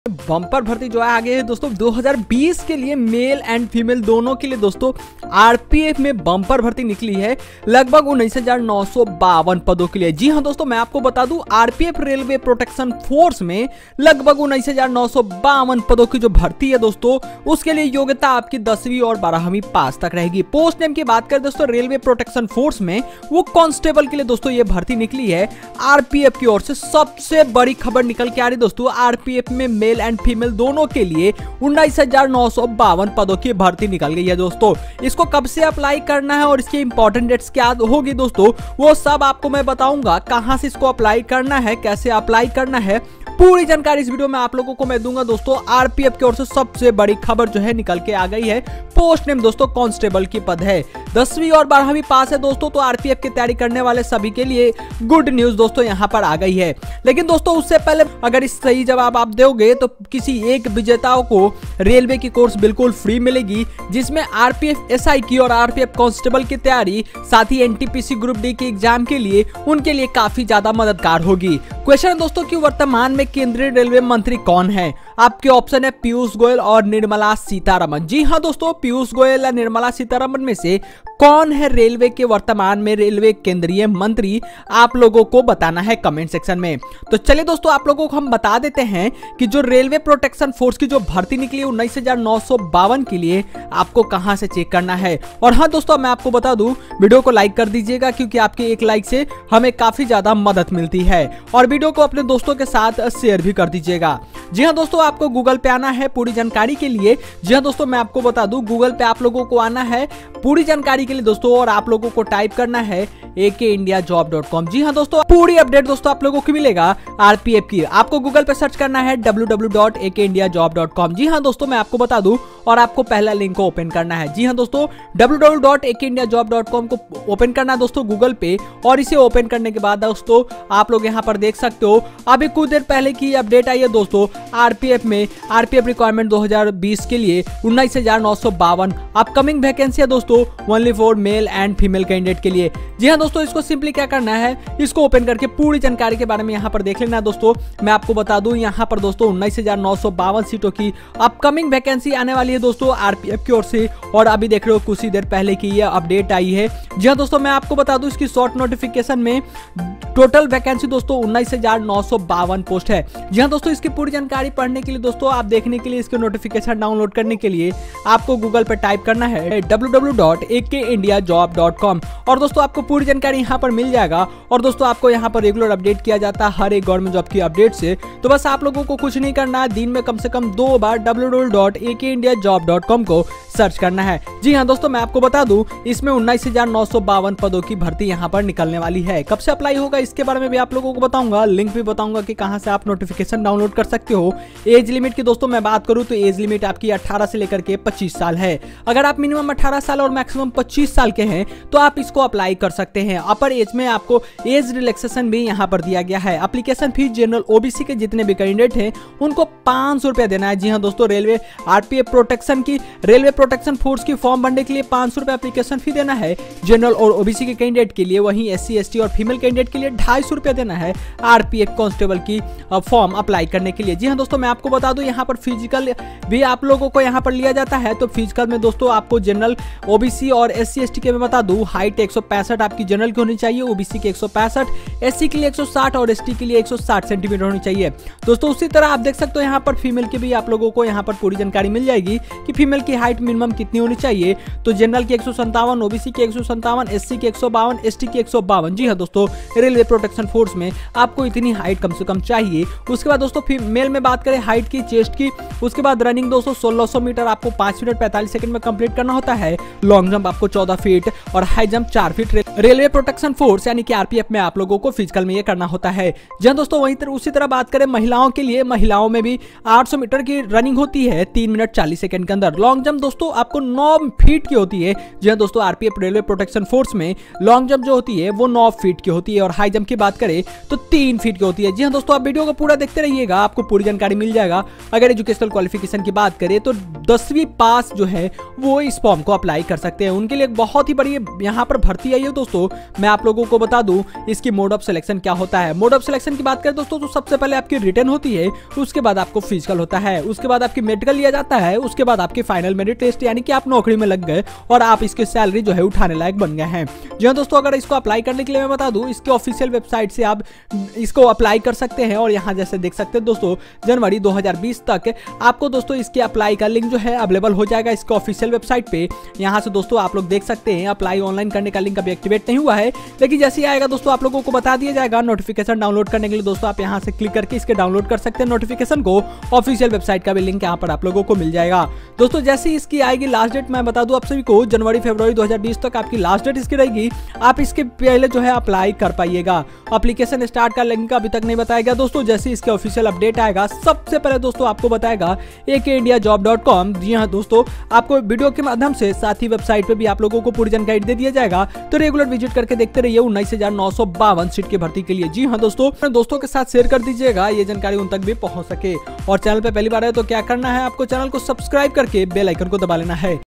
RPF constable Recruitment 2020 | Apply Online | RPF Vacancy 2020 | RPF bharti 2020 | Govt jobs
Railway Protection Security Force has Announced RPF Recruitment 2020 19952 vacancies for aspiring Male & Female candidates. Railway Recruitment Board has published job Notification for 19952 Vacancies for constable & SI Jobs. Intrested Railway Job aspirants can now apply for RPF Notification 2020 through online mode. However, Application forms for RPF Recruitment Constable Notification 2020 must be submitted before the last date Update Soon. Check additional information, such as the RPFJob. 2020 Eligibility, the details of the RPF Recruitment Constable Salary and others. However, the RPF Recruitment 2020 Online application Link is provided below

Thanku watching